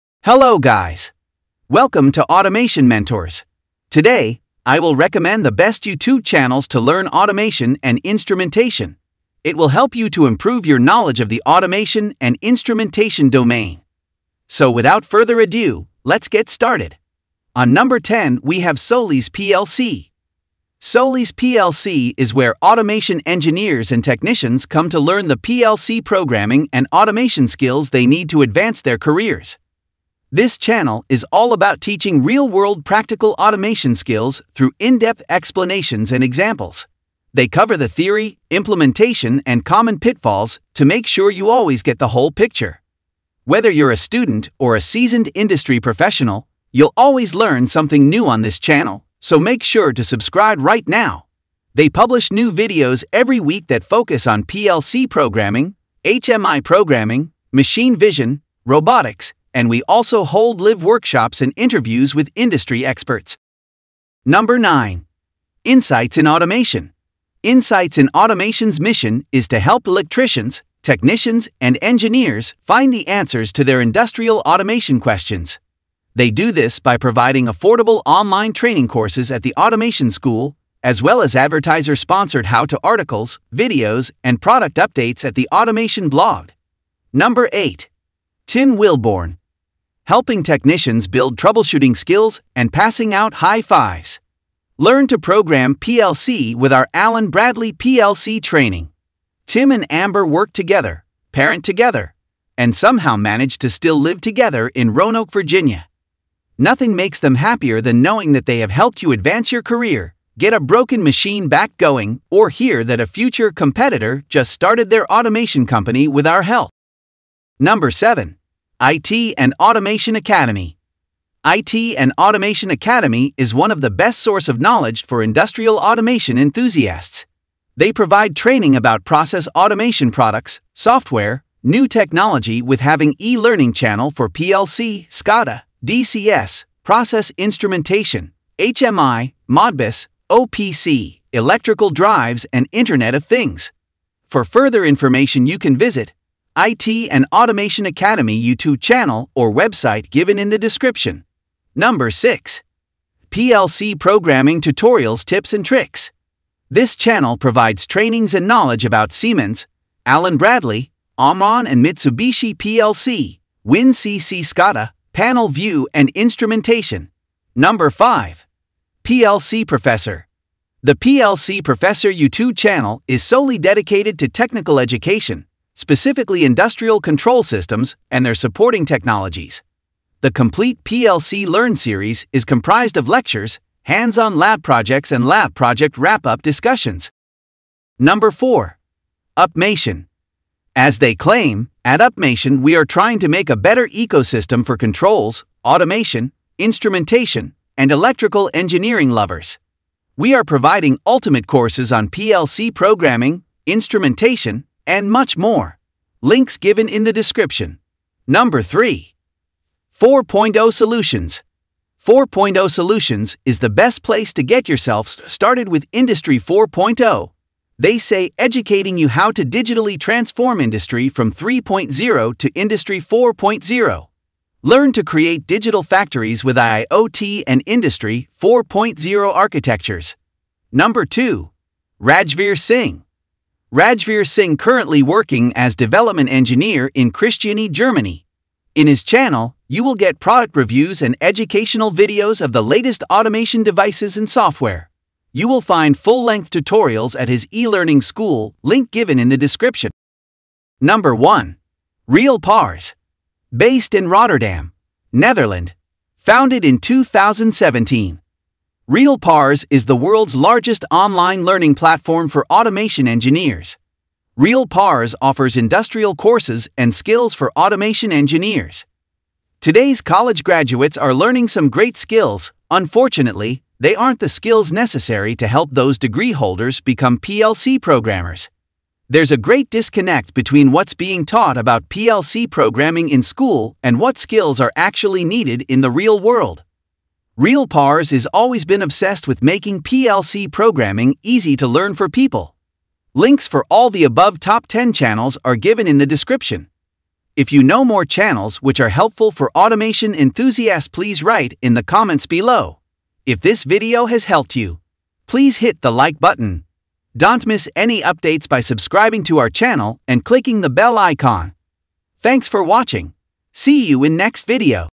Top 10 YouTube Channels to Learn PLC Programming for Free (2026)
Want to learn PLC Programming, SCADA, and Industrial Automation for free?

From Allen Bradley and Siemens tutorials to Industry 4.0 and IoT, these channels cover everything you need to start your career in automation without spending a dime.

⏱️ Timestamps:
00:00 - Intro: Best Way to Learn Automation
00:17 - 10. SolisPLC (PLC & HMI Training)
00:58 - 9. Insights In Automation (The Automation Blog)
01:27 - 8. Tim Wilborne (Allen Bradley Training)
01:58 - 7. IT and Automation Academy (Process Control)
02:34 - 6. PLC Programming Tutorials Tips and Tricks
02:49 - 5. PLC Professor (Industrial Control Systems)
03:10 - 4. Upmation (Control Panels & Instrumentation)
03:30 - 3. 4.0 Solutions (IIoT & Industry 4.0)
03:52 - 2. Rajvir Singh (Product Reviews & Tutorials)
04:12 - 1. RealPars (World's Largest Automation Platform)

Key Topics Covered:

Best Free Resources for PLC Programming

Where to learn Siemens TIA Portal & Allen Bradley Studio 5000

Top Industrial Automation Channels for Beginners

SCADA, DCS, and HMI Design Tutorials

To learn more about PLC, SCADA and DCS you can reach us through: